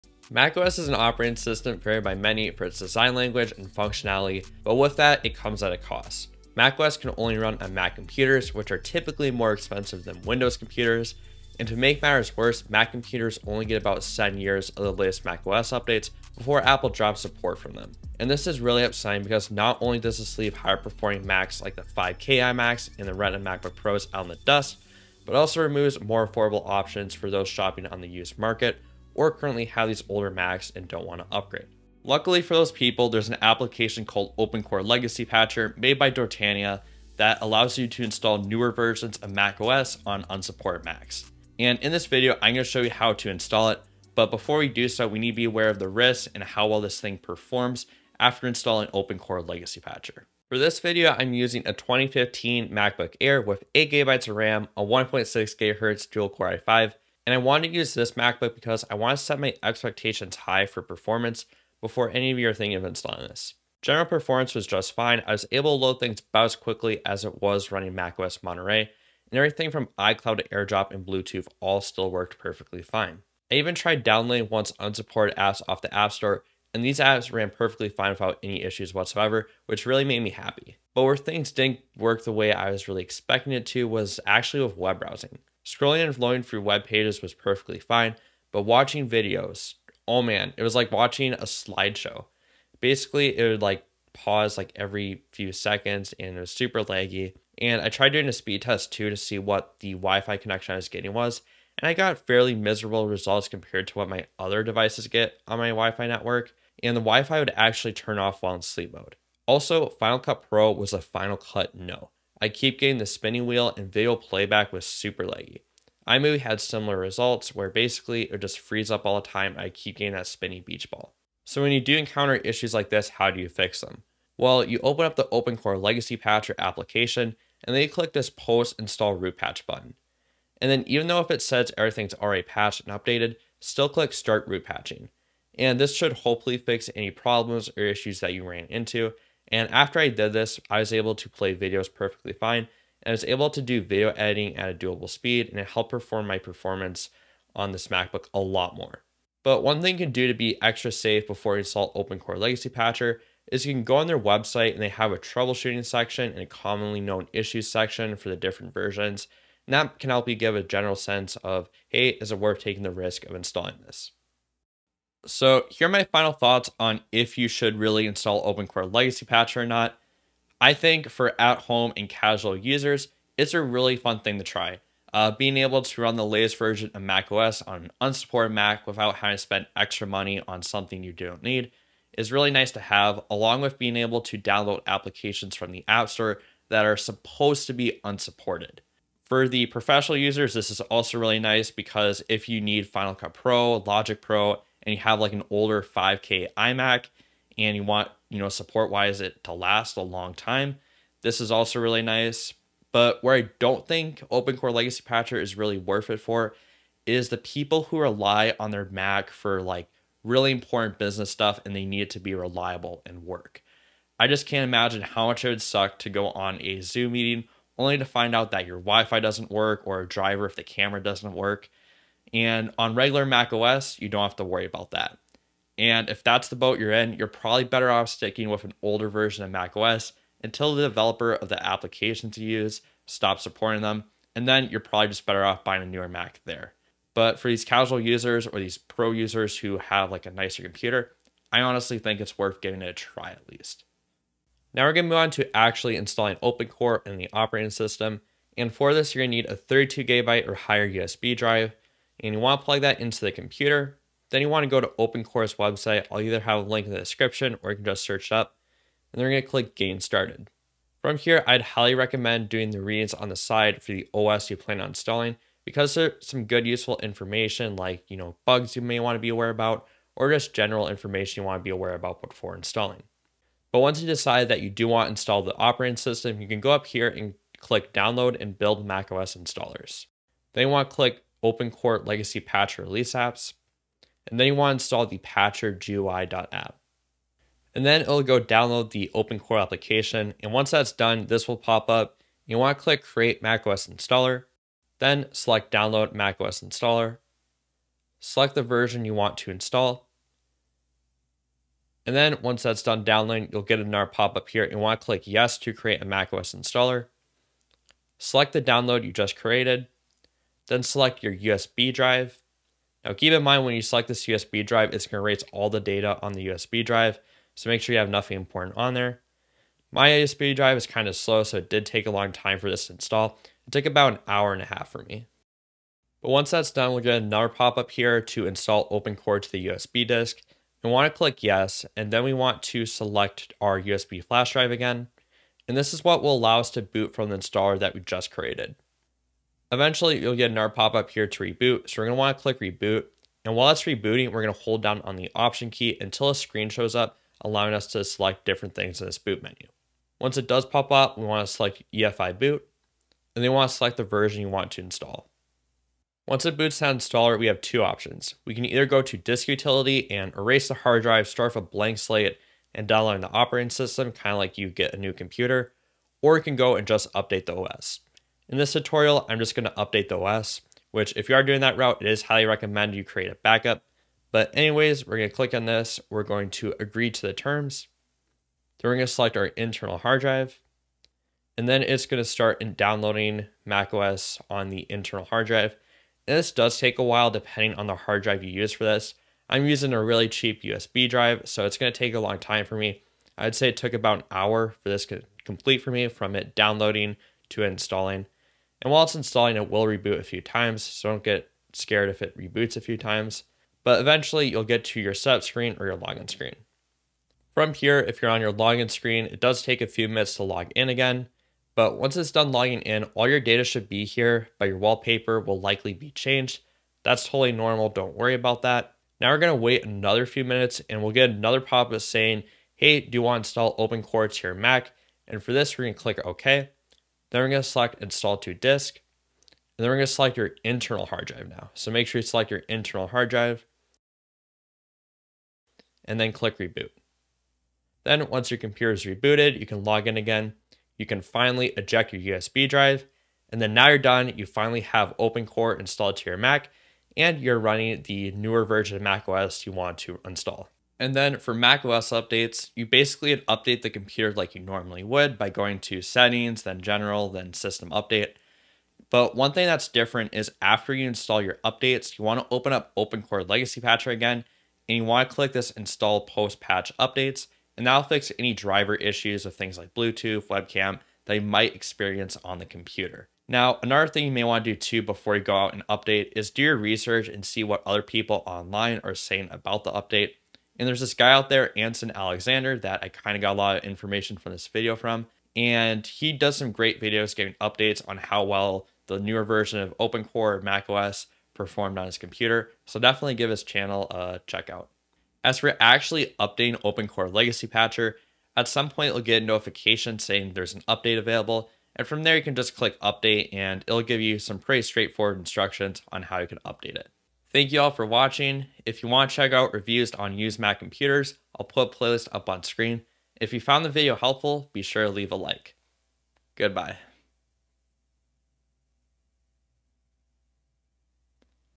How to Install Open Core Legacy Patcher | Full Guide!
Here's a tutorial on how to install open core legacy patcher with MacOS Sonoma or any version of MacOS you want to get on your unsupported mac.

My thoughts and opinions on these products are my own. Thank you :D

Chapters:
0:00 Before Installing?
4:04 How to install OPCLP
7:30 Updates